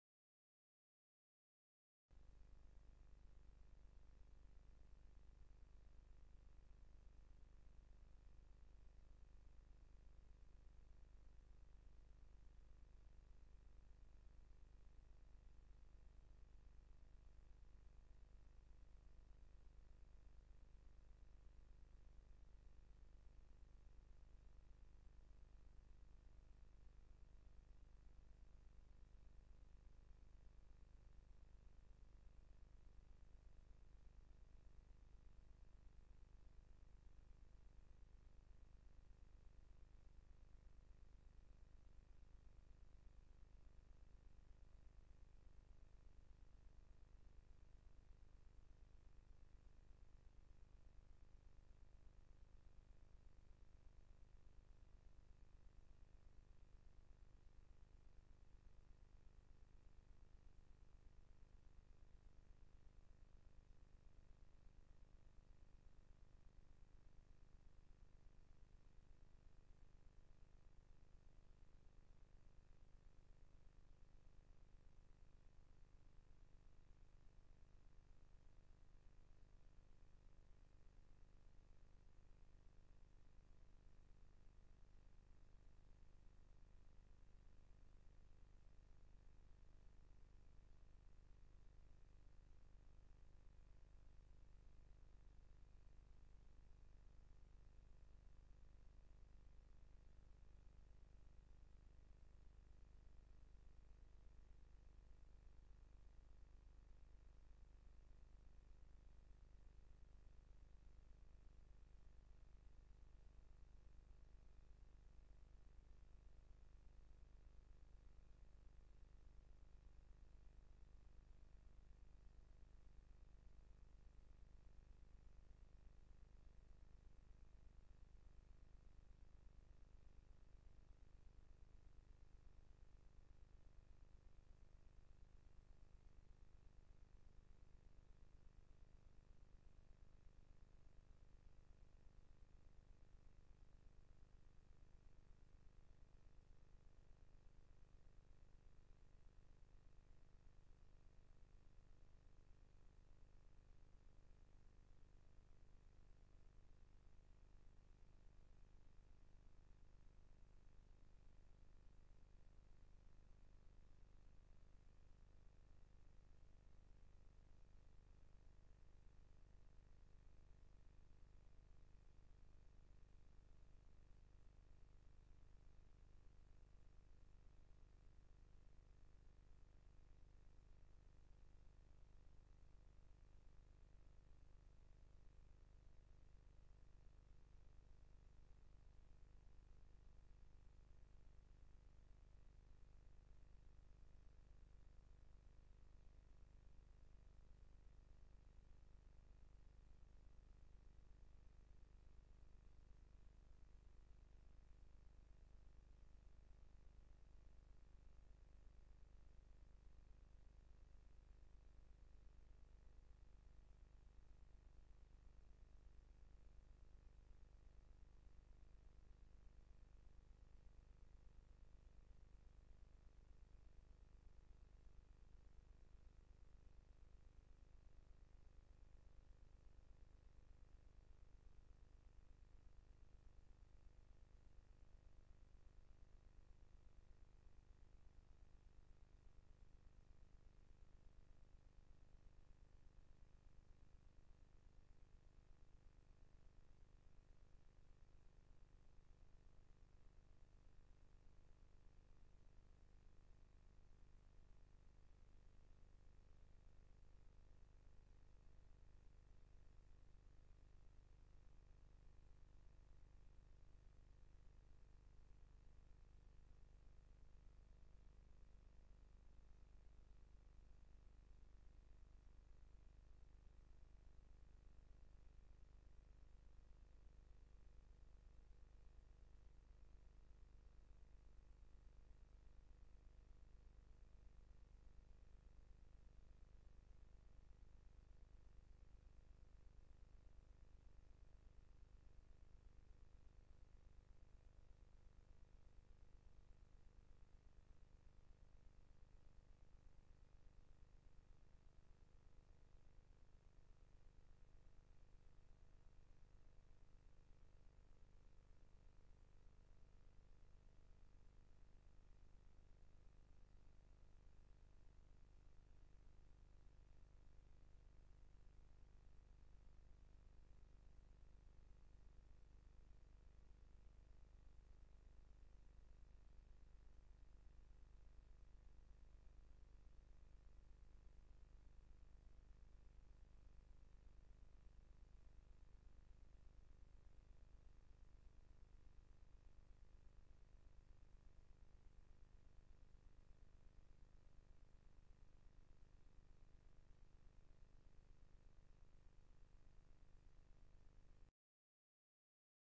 Ultrasonic Robotic Welding
A set of robots weld together a car's cock pit garniture.